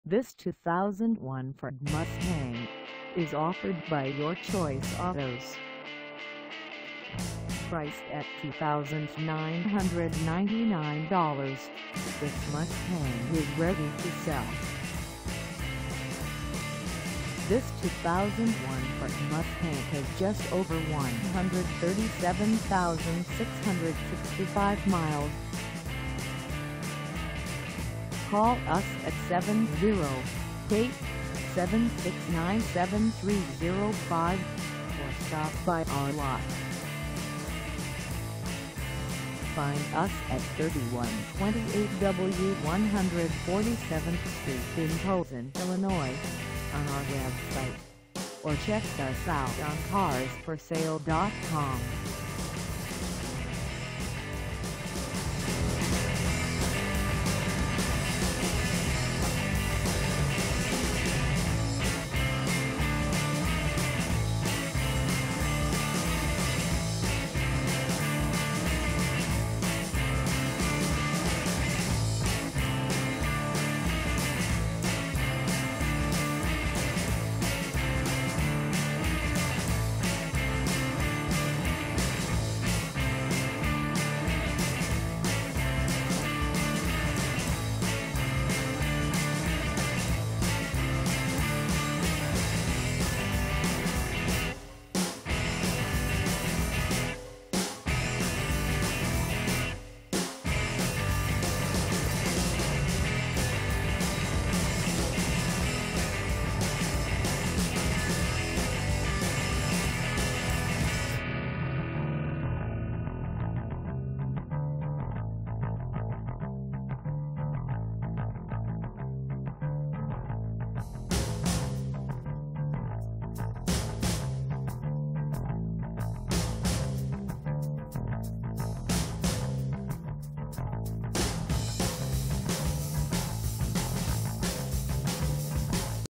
2001 Ford Mustang GT Deluxe 2dr Fastback for sale in Posen,
This 2001 Ford Mustang GT Deluxe 2dr Fastback is for sale in Posen, IL 60469 at Your Choice Autos.

Engine: 4.6L V8
Transmission: Automatic 4-Speed
Color: White
Mileage: 137665
Contact Your Choice Autos 3128 W. 147th St Posen, IL 60469 to test drive this 2001 Ford Mustang GT Deluxe 2dr Fastback today.